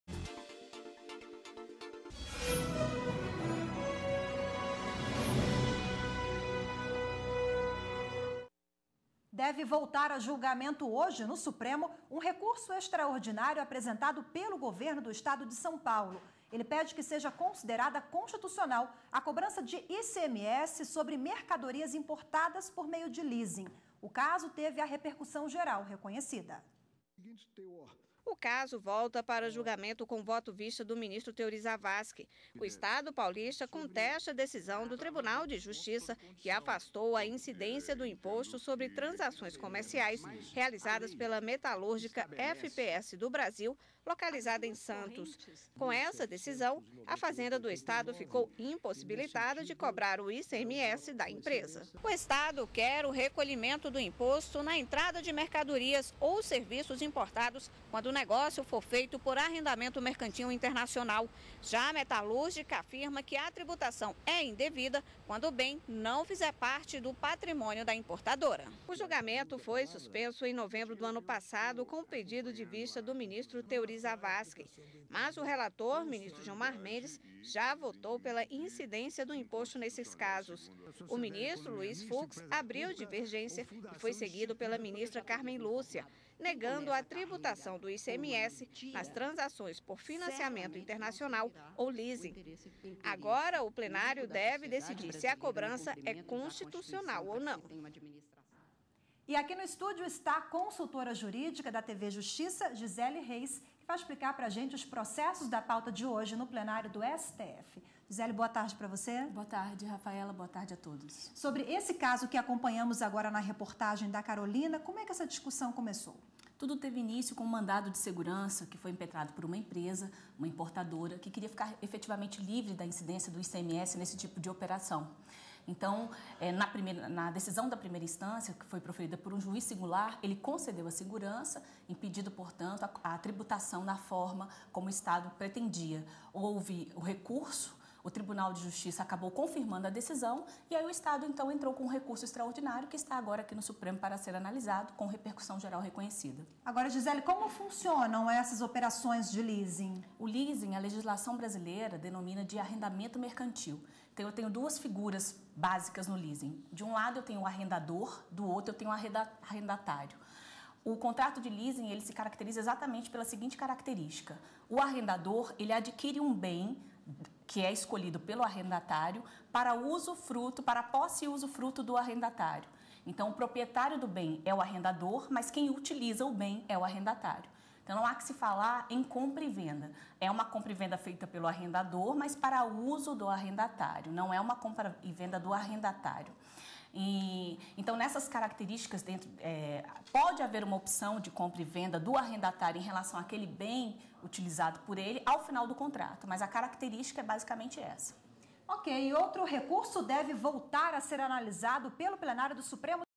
Recurso extraordinário (11/09/14)
Deve voltar a ser julgado hoje (11) no Supremo um recurso extraordinário apresentado pelo governo de São Paulo (SP). Ele pede que seja considerada constitucional a cobrança de ICMS sobre mercadorias importadas por meio de leasing. O caso teve repercussão geral reconhecida.